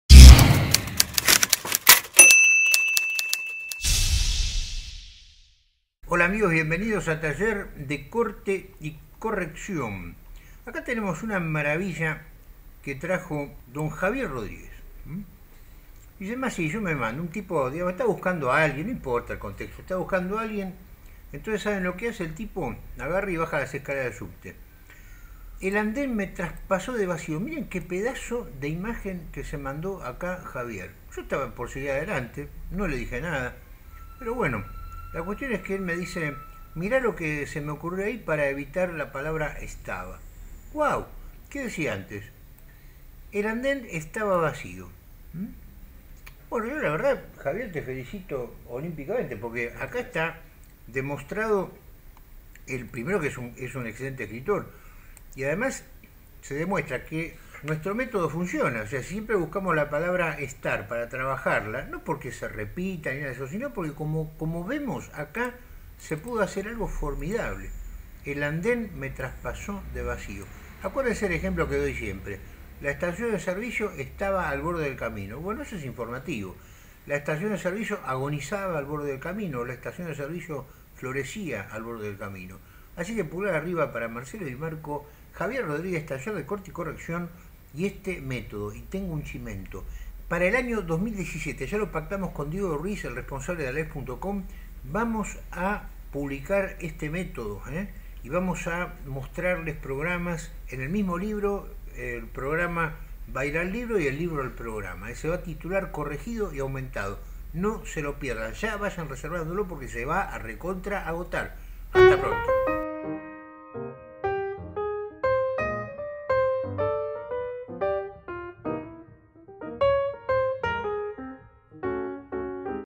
Cómo ganar una imagen poética (método TCyC) /Naranja #159
Mirá qué se puede lograr cuando, después de identificar en el texto uno de nuestros verbos favoritos, lo trabajamos como Dios manda. ¡No te pierdas esta nueva Naranja del TCyC, y muchas gracias por likearla y compartirla!